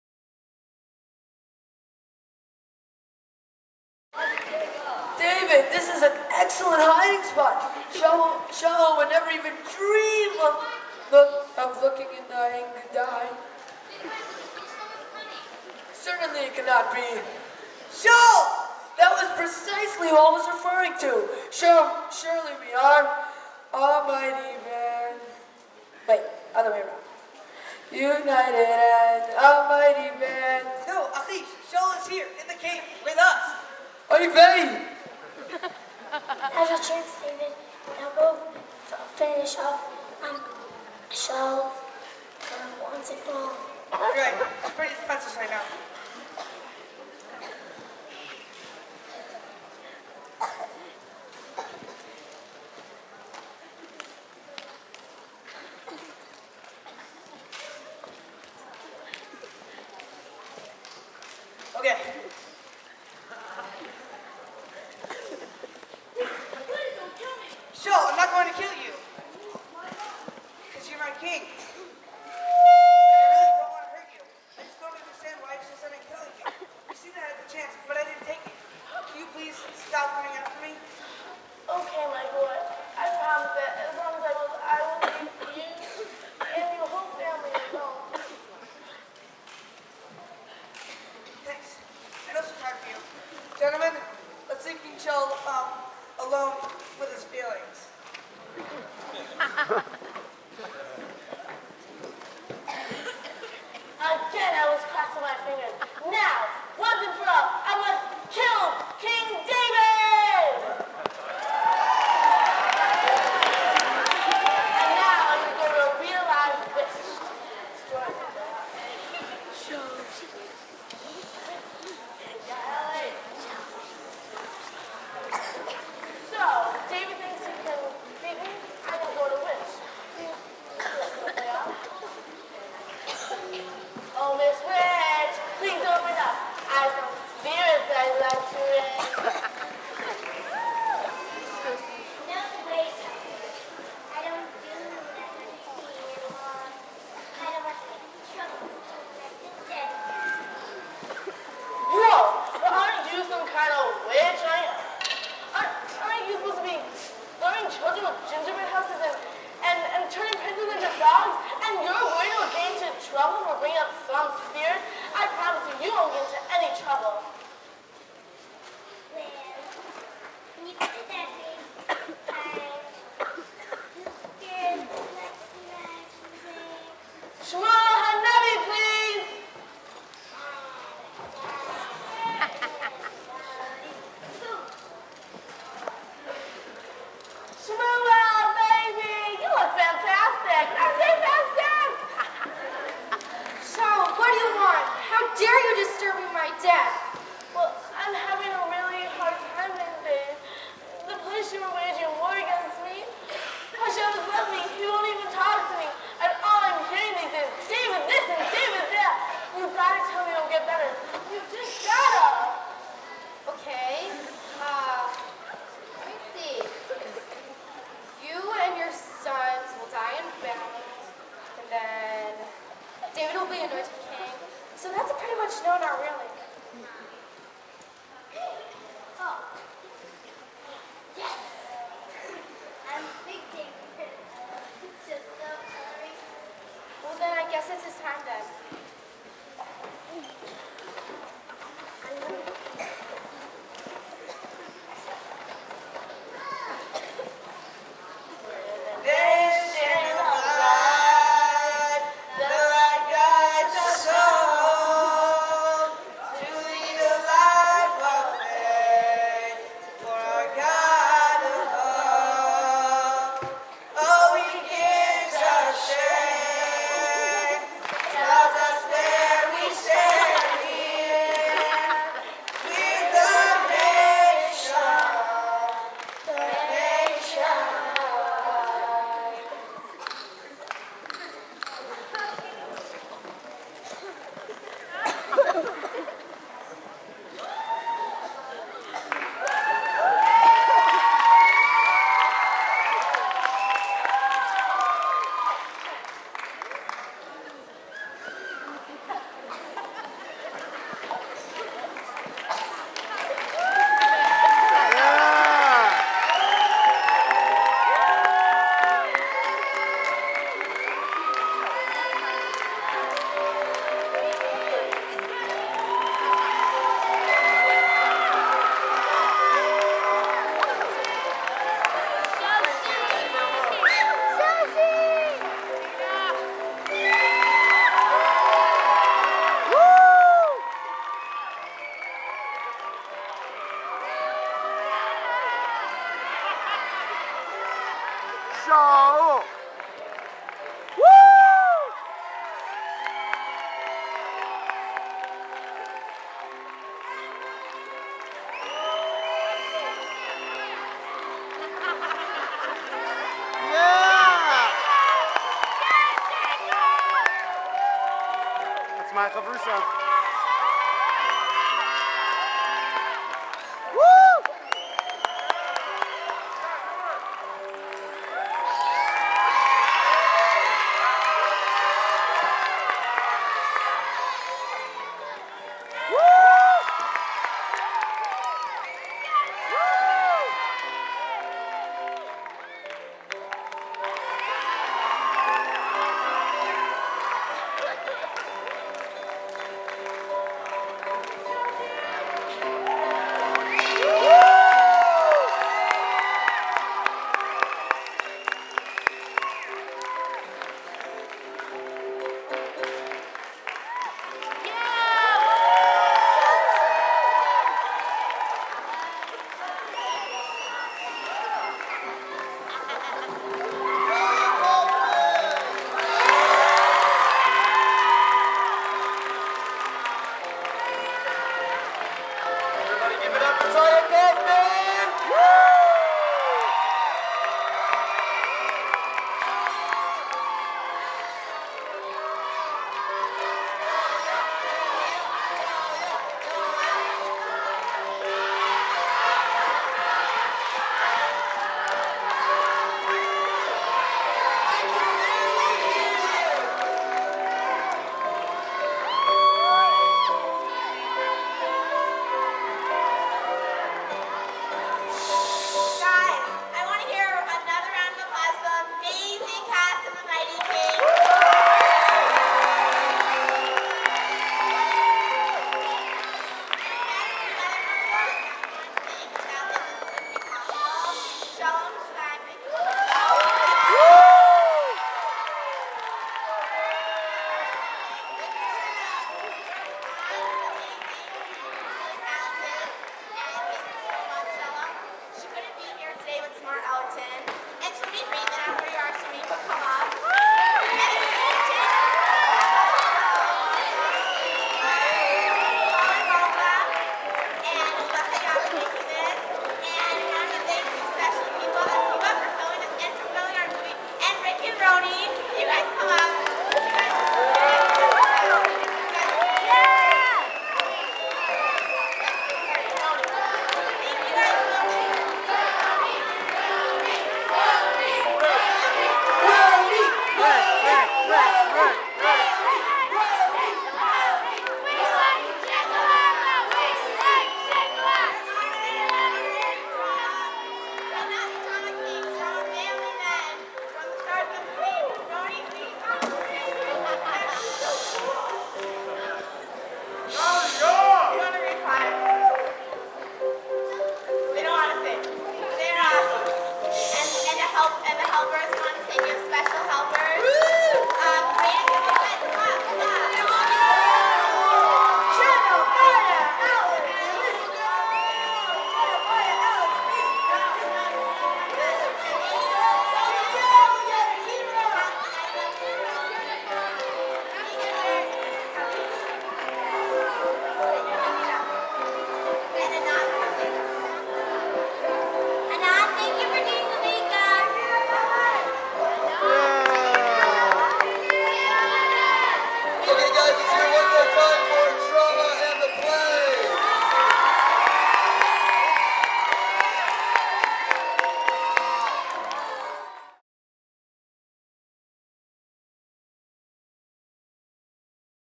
Part 4/4 - The Mighty King Drama Play Camp Stone 2009 First Session
Camp Stone's Drama Chug put on a wonderful play on the last full day of camp. It was called "The Mighty King." Inspired by The Lion King, it tells the story of David and Saul. Enjoy!